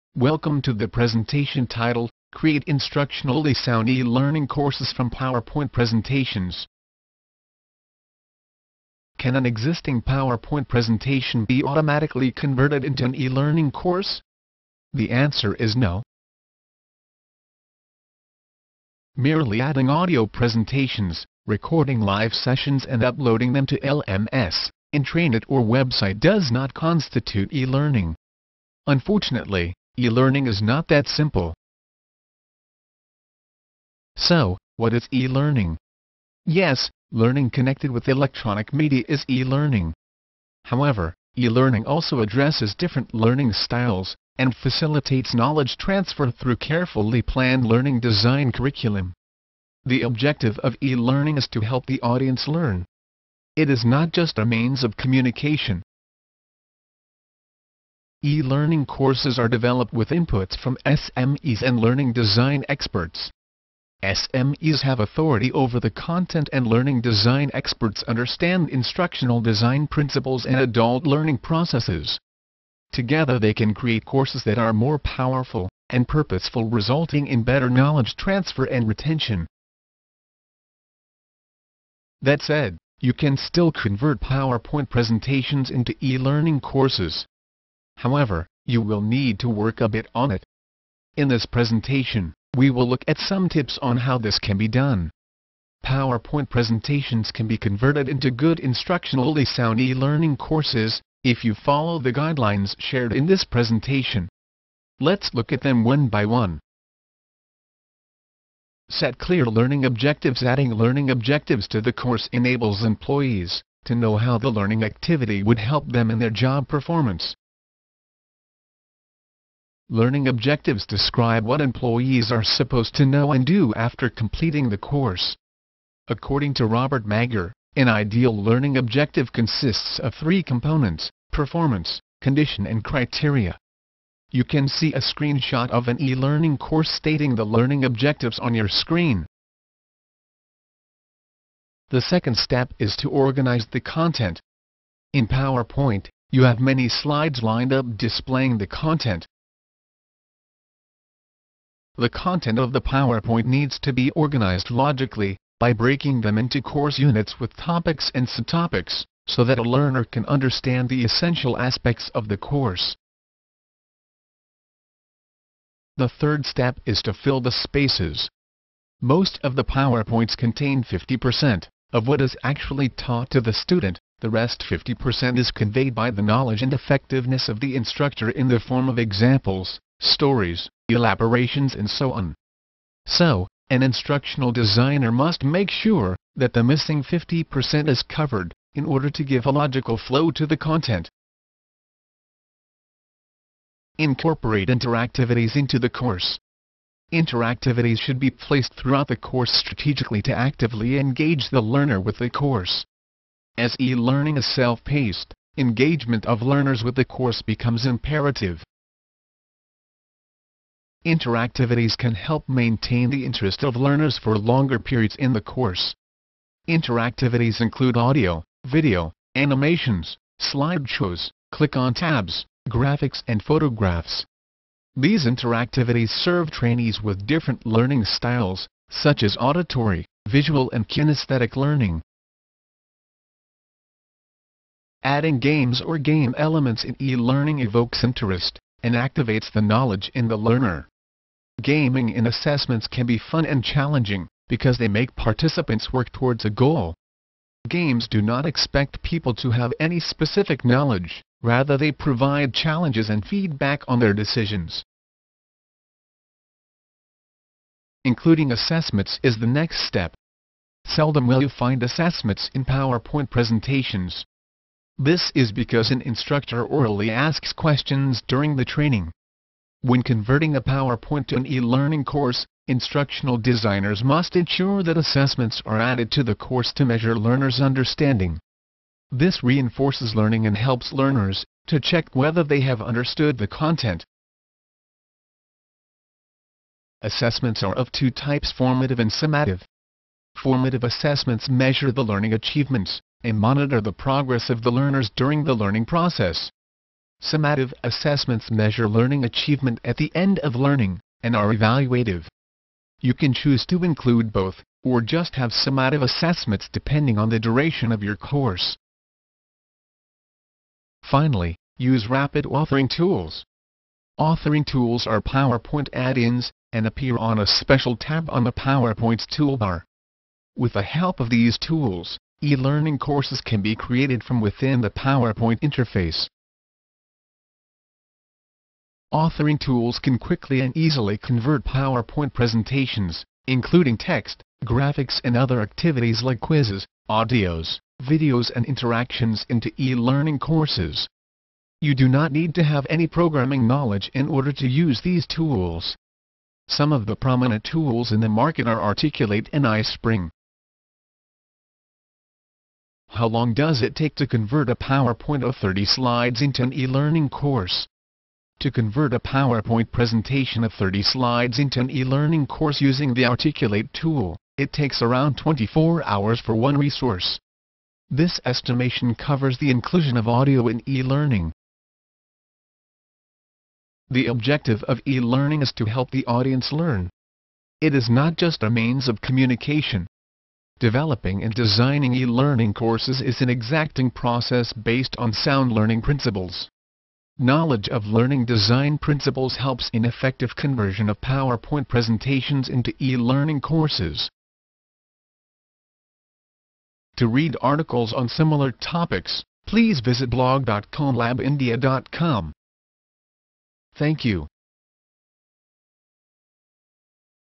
Transforming Classroom PowerPoints to E-Learning? Learn How!
Converting PowerPoint presentations into engaging e-learning courses has never been easier! In this  video, we'll show you step-by-step how to transform your classroom-based training materials into dynamic online courses that captivate and engage your audience. Say goodbye to traditional PowerPoint presentations and Hello!! to interactive and immersive e-learning experiences. Don't miss out, watch now and uncover the secrets to elevating your training programs to the next level.